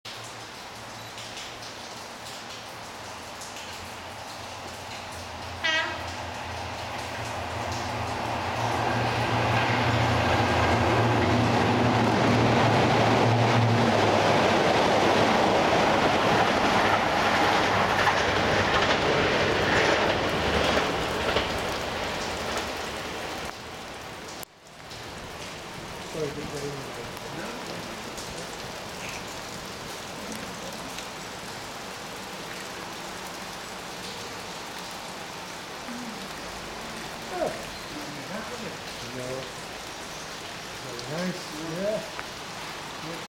D9009 hauling 55002 55019 in Hebden Bridge
Deltic D9009 hauls Deltics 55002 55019 in convoy though Hebden Bridge station on 25th September 2012 at about 1015h, en route to York (initially). The three engines had been at the East Lancashire Railway for their Deltic Gathering.